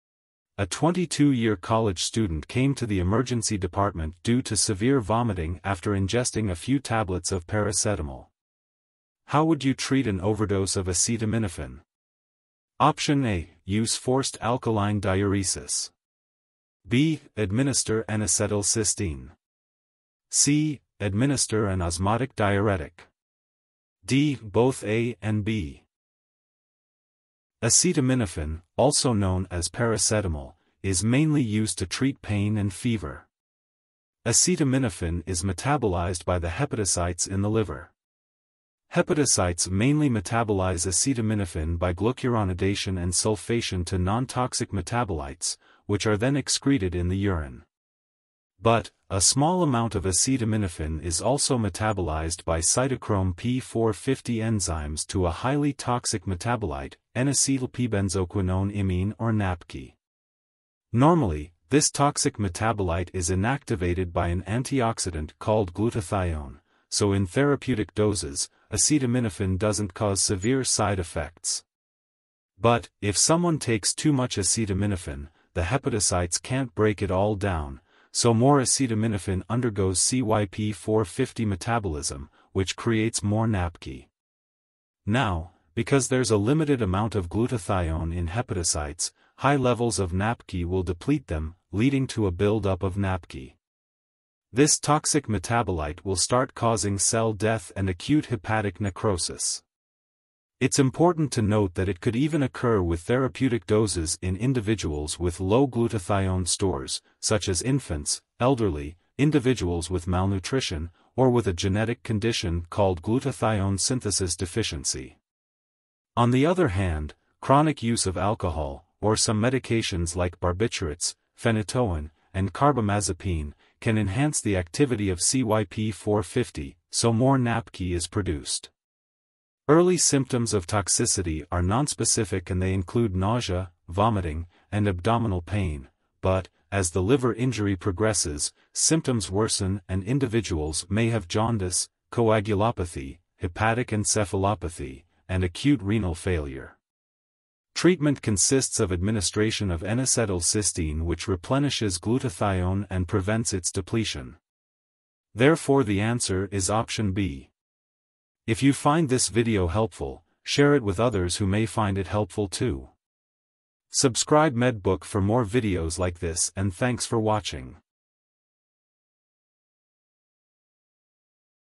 DHA, HAAD PHARMACIST EXAM Q&A PART 38
This Q&A series provides new pattern questions, including case studies and explained answers to the DHA HAAD pharmacist exam. It is designed to help pharmacists prepare for the exam and pass it successfully.